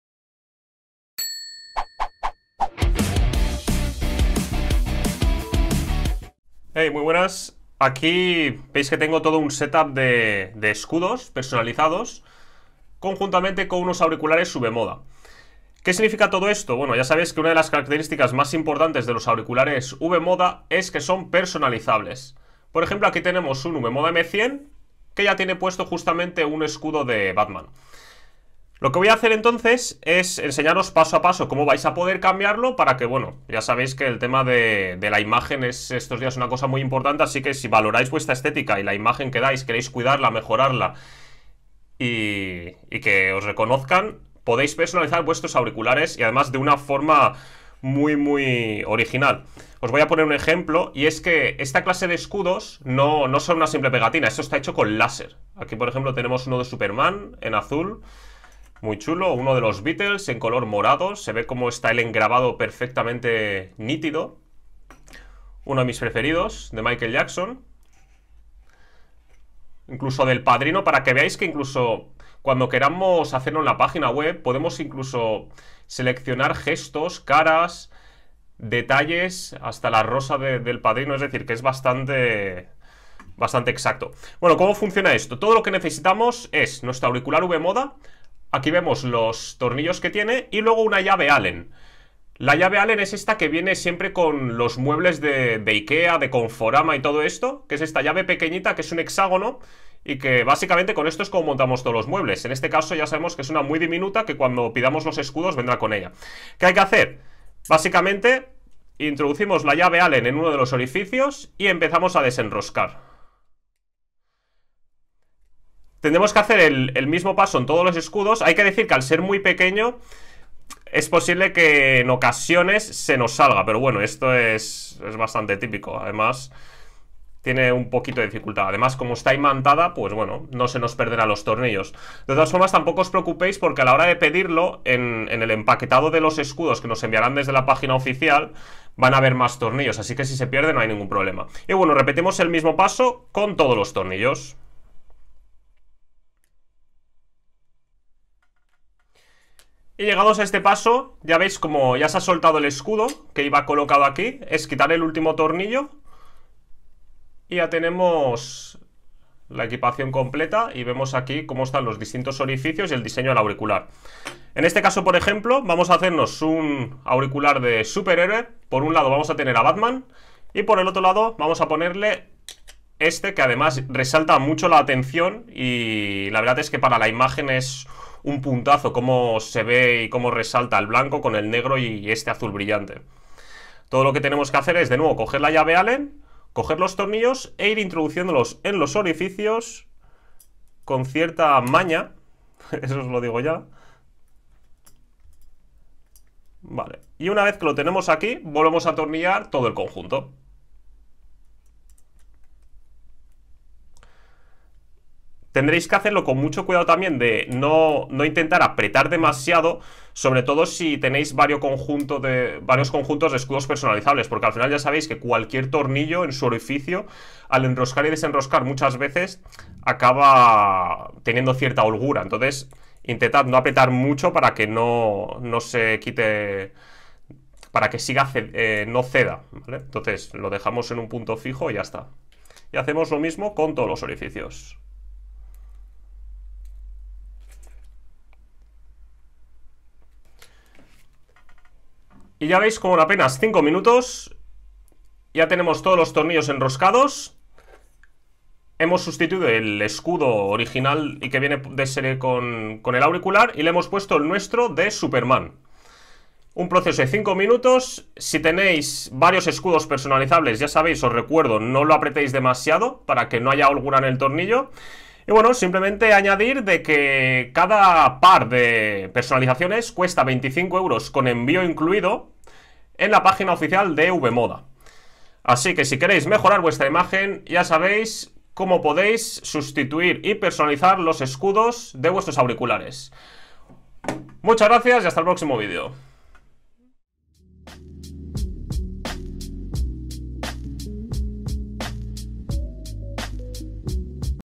Cómo cambiar shields/escudos personalizados de V-MODA
En este vídeo os enseñamos a cómo cambiar los escudos o shields de vuestros V-MODA Crossfade LP2, M-100 y Wireless.

Debéis saber que son personalizables, por lo que podréis poner el contenido que queráis. Podéis diseñar los escudos o shields directamente desde la web de V-MODA

Tenemos en stock toda la gama de auriculares V-Moda en nuestra web